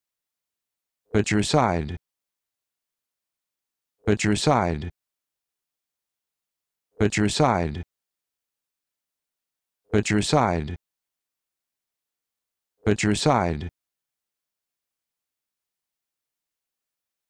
How to Pronounce Patricide
This video shows you how to pronounce Patricide